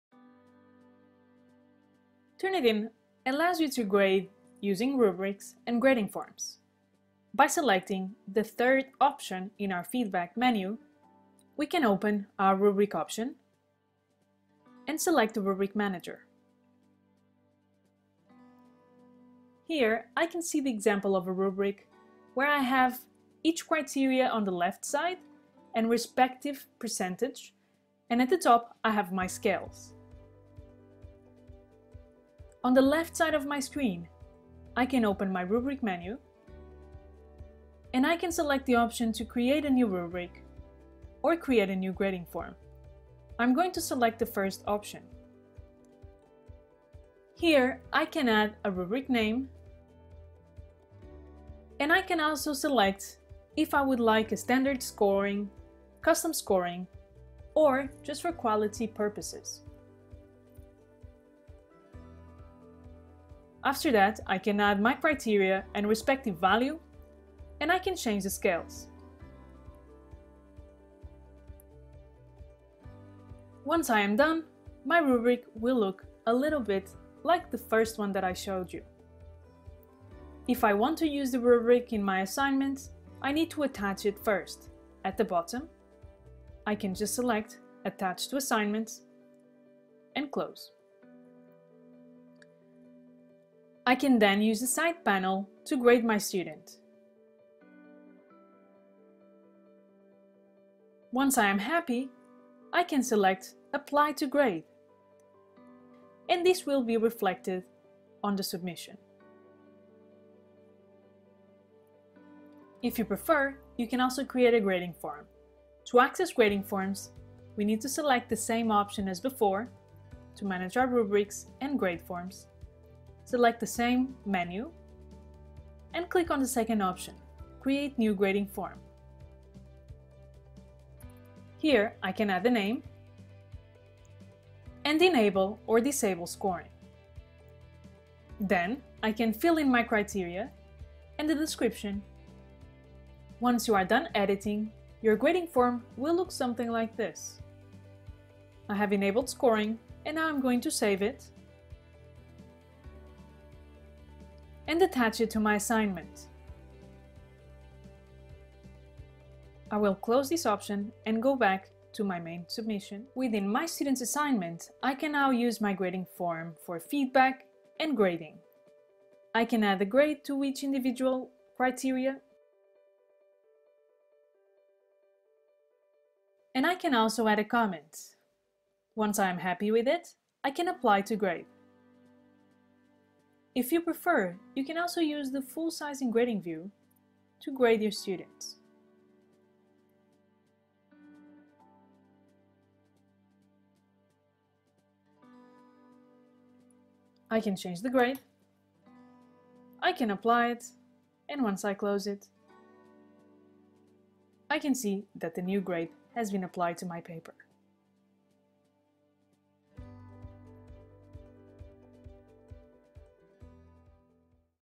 How to Use Rubrics and Grading Forms, Part 2
These VidBITs shows instructors where to find and how to use the features of the Rubrics and Grading Forms in Feedback Studio, including how to create rubrics and grading forms, the differences between them, and how to access Turnitin's genre, grade level, and content area rubrics.